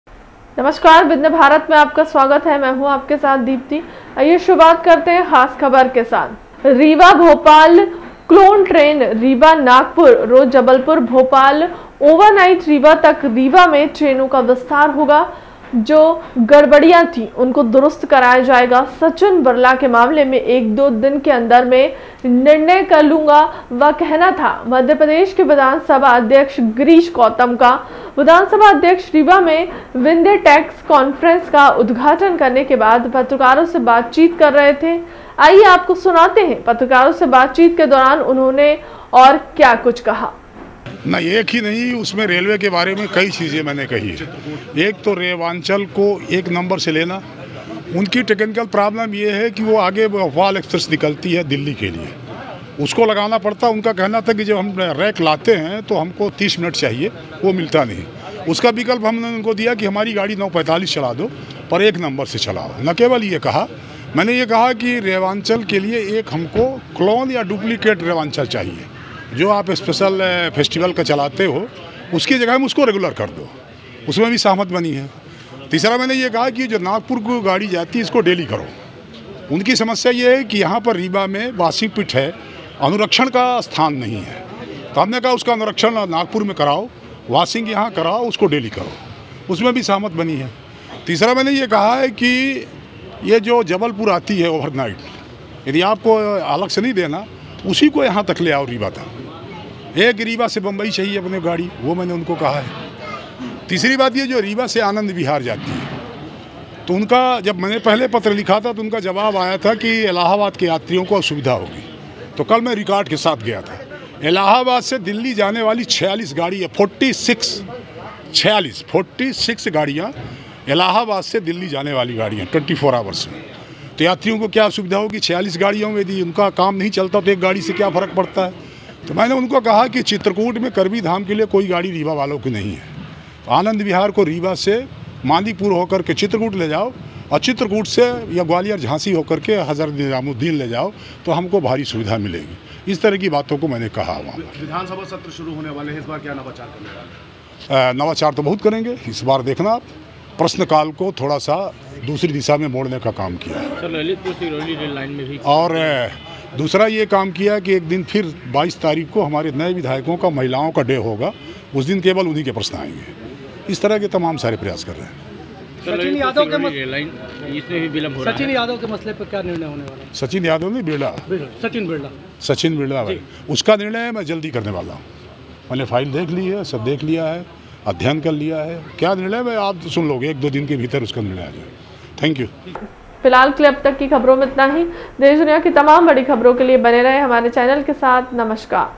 भोपाल ओवरनाइट तक रीवा में ट्रेनों का होगा विस्तार
रीवा भोपाल क्लोन ट्रेन रीवा नागपुर रोज जबलपुर भोपाल ओवरनाइट रीवा तक रीवा में ट्रेनों का विस्तार होगा जो गड़बड़ियां थी उनको दुरुस्त कराया जाएगा सचिन बिरला के मामले में एक-दो दिन के अंदर मैं निर्णय कर लूंगा यह कहना था मध्य प्रदेश के विधानसभा अध्यक्ष गिरीश गौतम का विधानसभा अध्यक्ष रीवा में विंध्या टैक्स कॉन्फ्रेंस का उद्घाटन करने के बाद पत्रकारों से बातचीत कर रहे थे आइए आपको सुनाते पत्रकारों से बातचीत के दौरान उन्होंने और क्या कुछ कहा